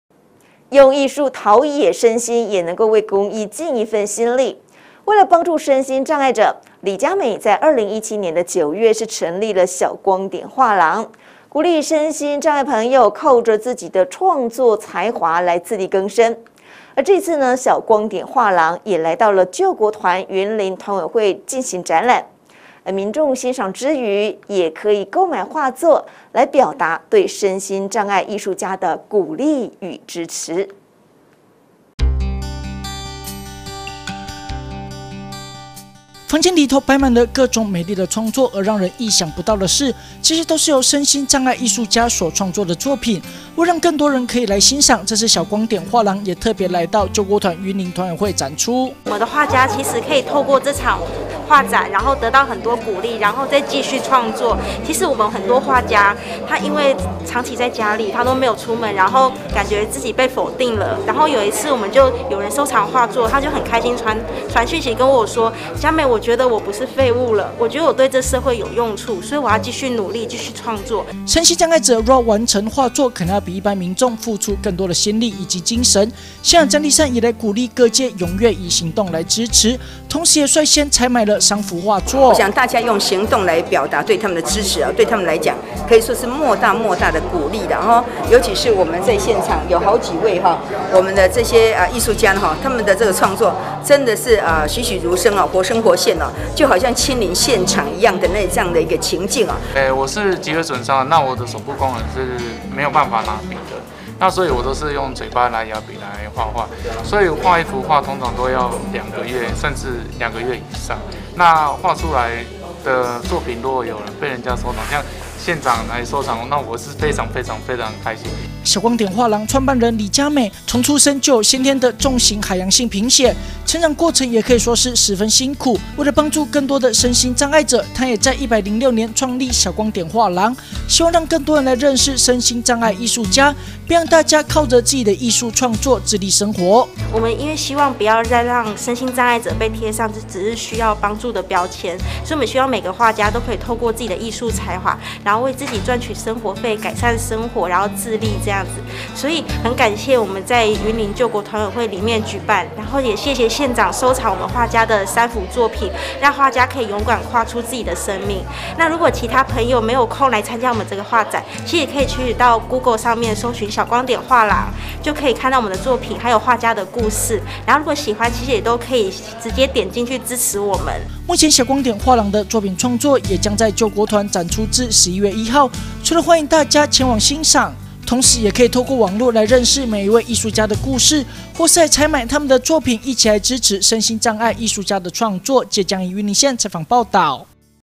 小光點藝術畫展 展現身心障不屈生命力
用藝術陶冶身心，也能為公益盡一份心力！為了幫助身心障礙者，李佳鎂在2017年9月，成立了小光點畫廊，鼓勵身心障礙者，靠著自己的創作才華自力更生。這次小光點畫廊也來到救國團雲林團委會展覽，民眾欣賞之餘，也可以購買畫作，來表達對身心障礙藝術家的鼓勵與支持。